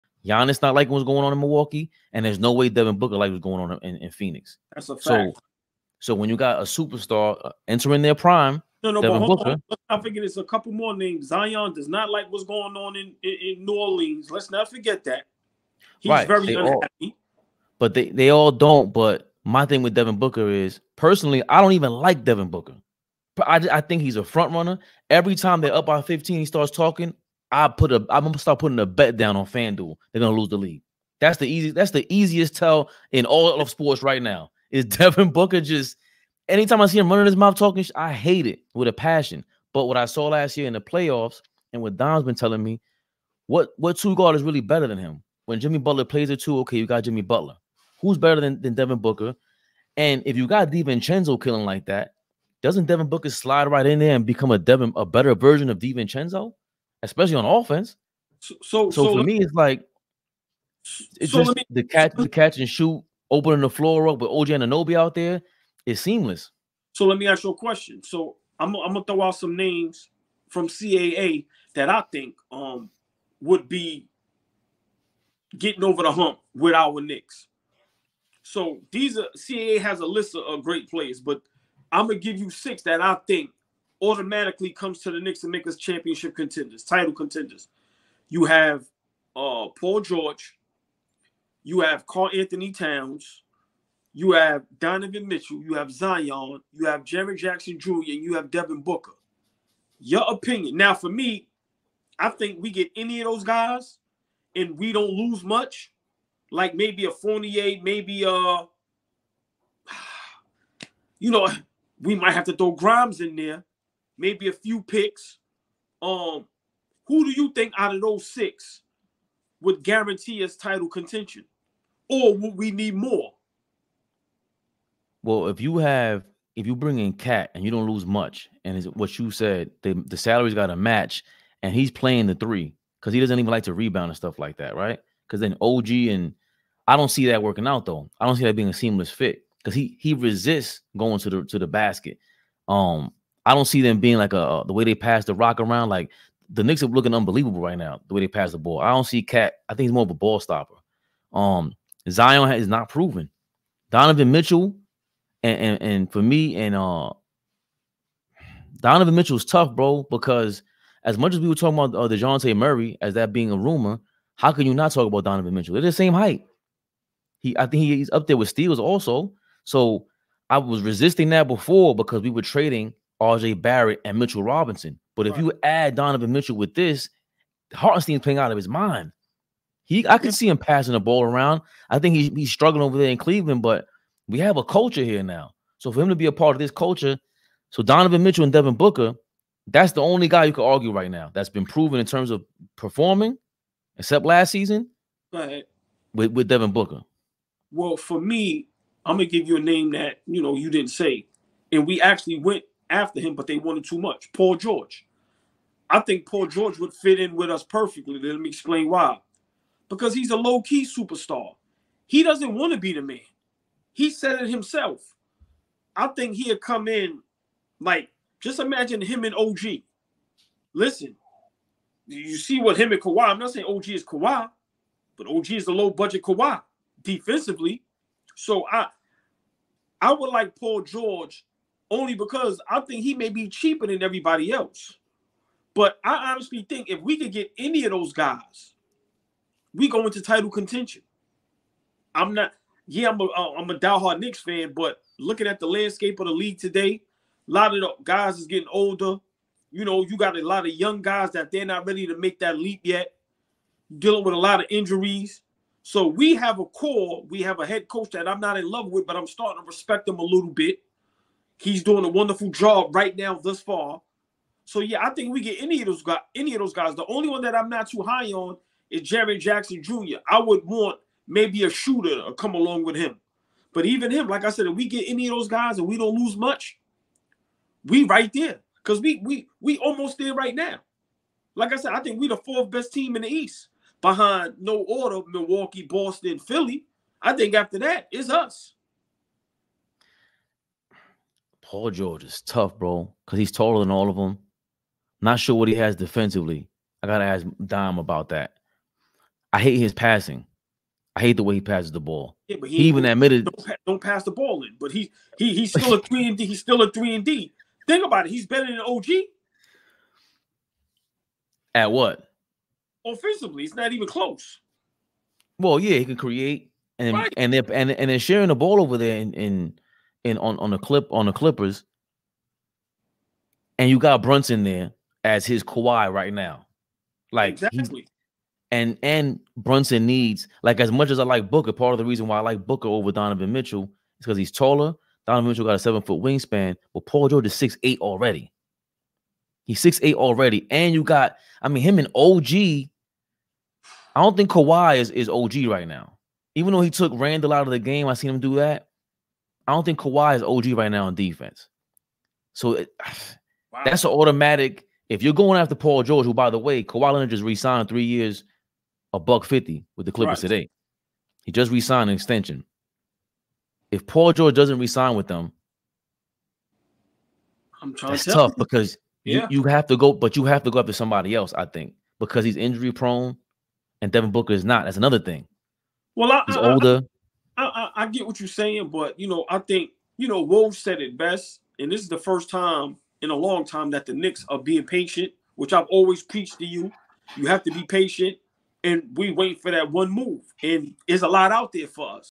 Knicks Get Devin Booker and Paul George Call... | Knicks Rehab
AllBall TV Presents Knicks Rehab

The Knicks Rehab discussion revolves around dissatisfaction among NBA players like Giannis in Milwaukee, Zion in New Orleans, and Devin Booker in Phoenix. Devin Booker receives criticism for being perceived as a front-runner, with a betting strategy mentioned based on his behavior during games.

The Knicks Rehab conversation shifts to potential players from the CAA list who could elevate the New York Knicks to championship contenders, including Paul George, Karl-Anthony Towns, Donovan Mitchell, Zion Williamson, Jaren Jackson Jr., and Devin Booker. Evaluation of players focuses on their fit with the Knicks, with particular attention to Donovan Mitchell and Paul George.

The importance of patience and waiting for the right move is emphasized, acknowledging the Knicks as the fourth-best team in the Eastern Conference behind Milwaukee, Boston, and Philadelphia.

All Ball TV Contacts;
Youtube Channels